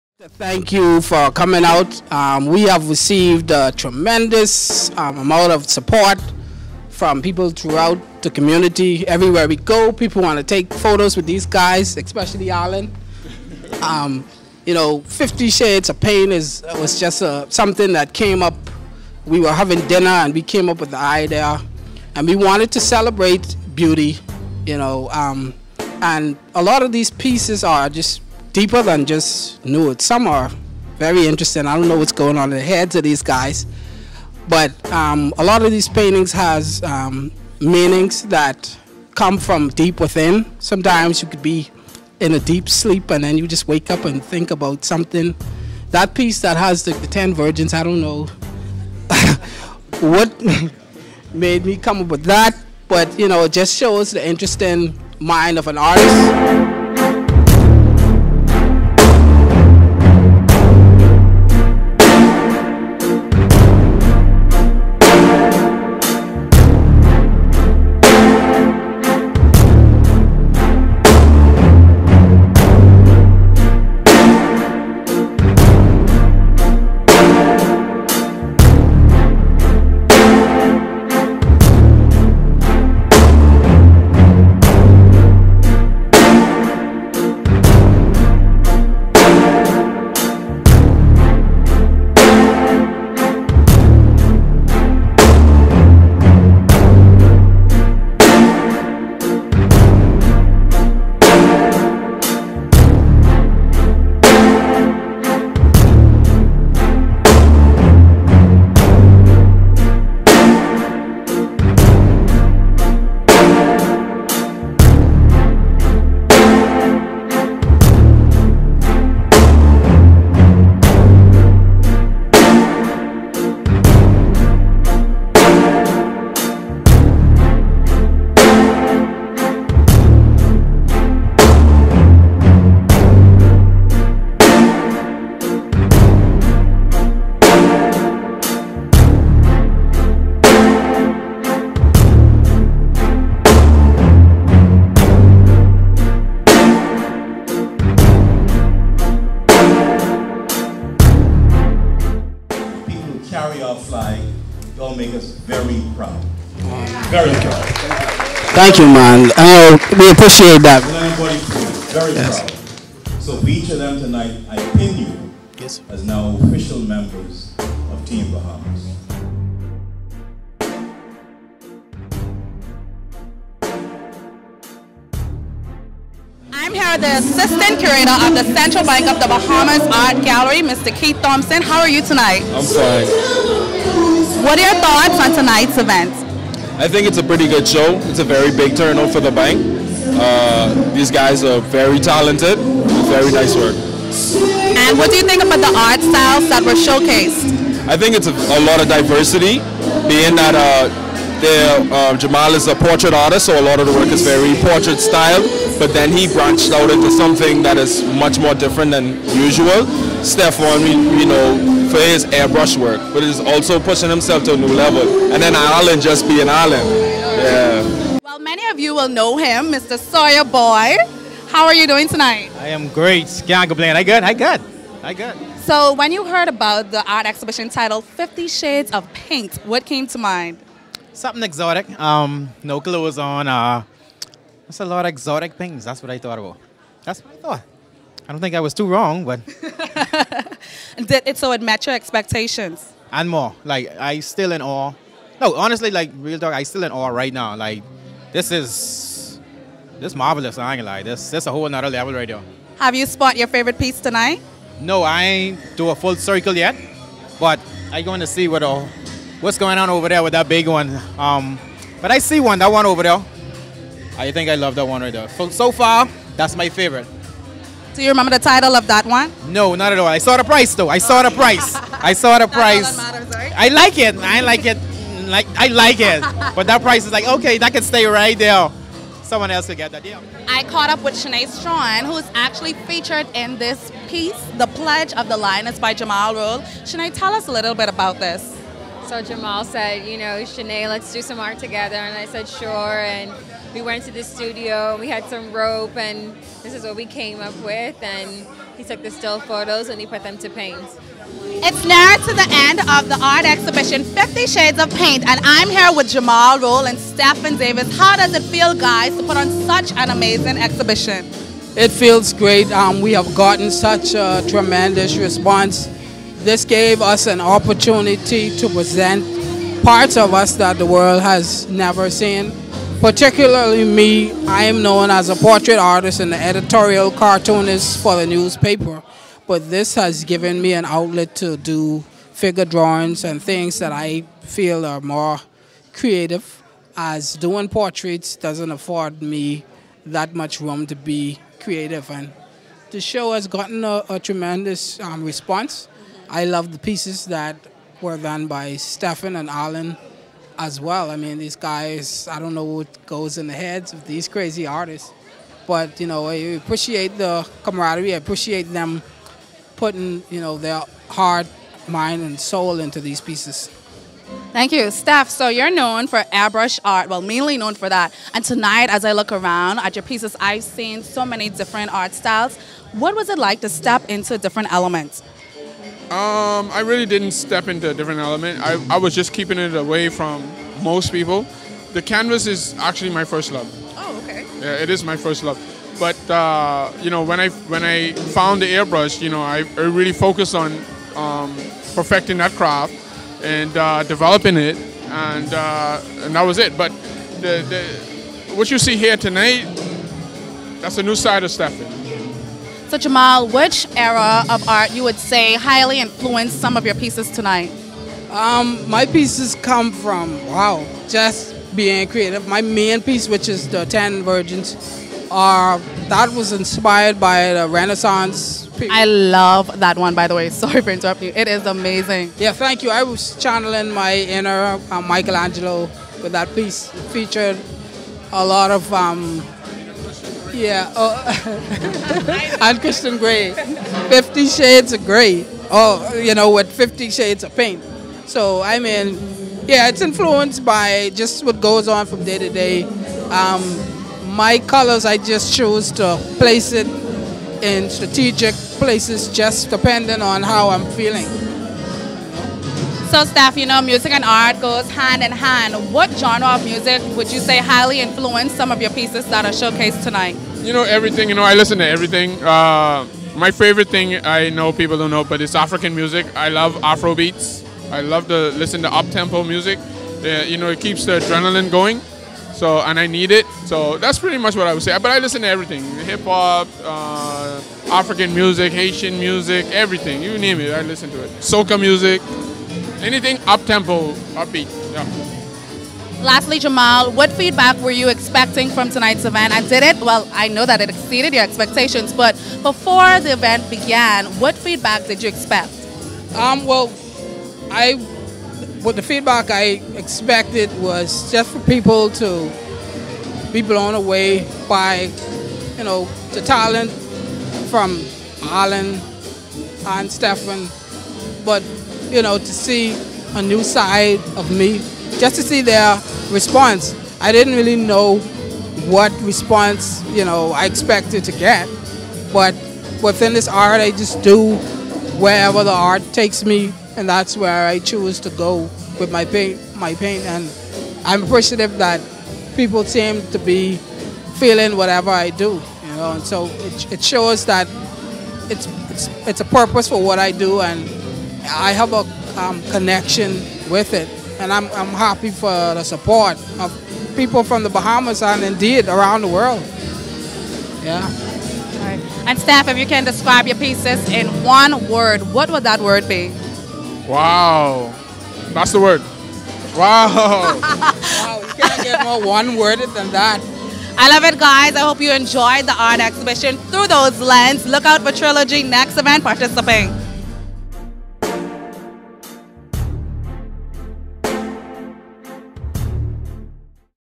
3RILOGY Presents: Fifty Shades of Paint
Jamaal Rolle, Stefan Davis & Allan Pachino Wallace, better known as 3rilogy, held an art exhibition at the Central Bank Of the Bahamas Art Gallery titled 50 Shades of Paint. The exhibition was designed to educate, entertain and give viewers an engaging experience that explores beauty in the characteristics of humankind.